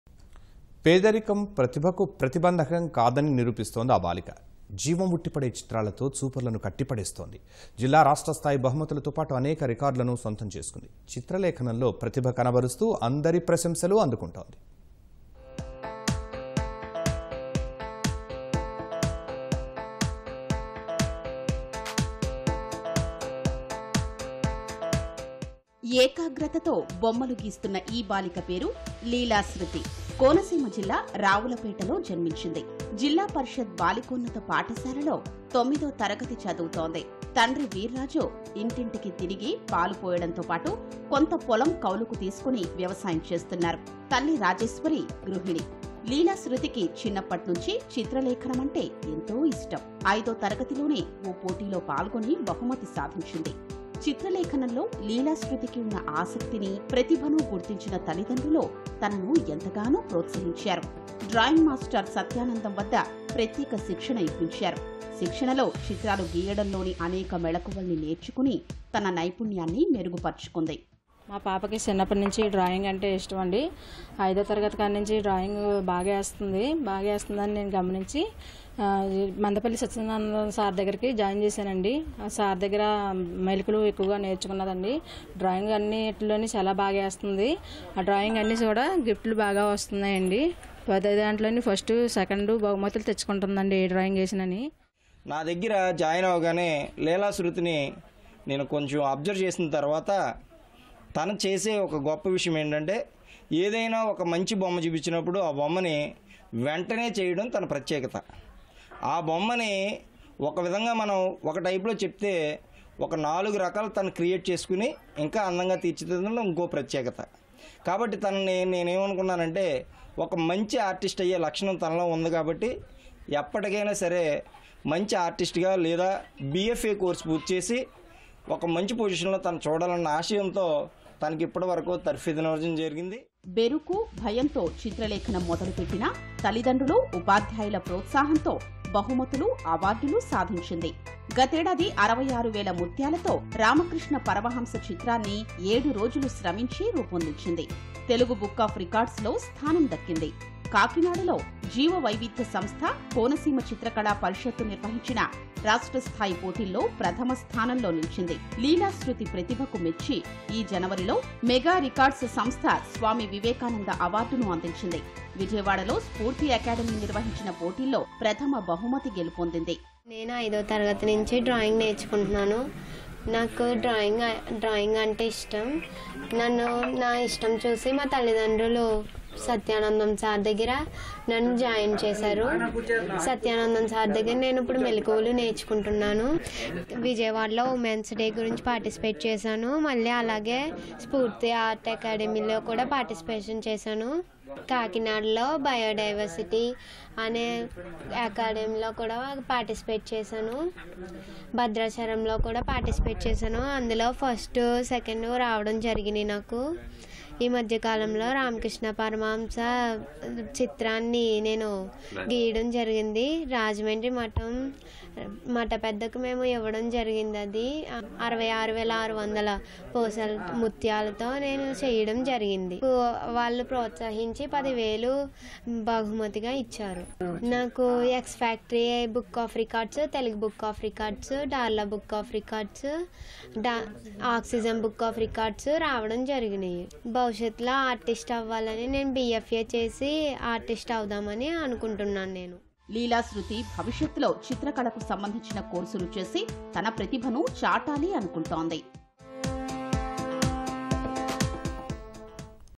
చిత్రలేఖనంలో అందరినీ ఆకట్టుకుంటున్న లీలా శ్రుతి | Artist Leela Sruthi Attracts by His Paintings
పేదిరికం ప్రతిభకు ప్రతిబంధకం కాదని నిరూపిస్తోంది ఆ బాలిక. జీవం ఉట్టిపడే చిత్రాలతో చూపరులను కట్టిపడేస్తోంది. జిల్లా, రాష్ట్రస్థాయి బహుమతులతోపాటు అనేక రికార్డులూ సొంతం చేసుకుంది. చిత్రలేఖనంలో ప్రతిభ కనుబరుస్తూ అందరి ప్రశంసలు అందుకుంటోంది.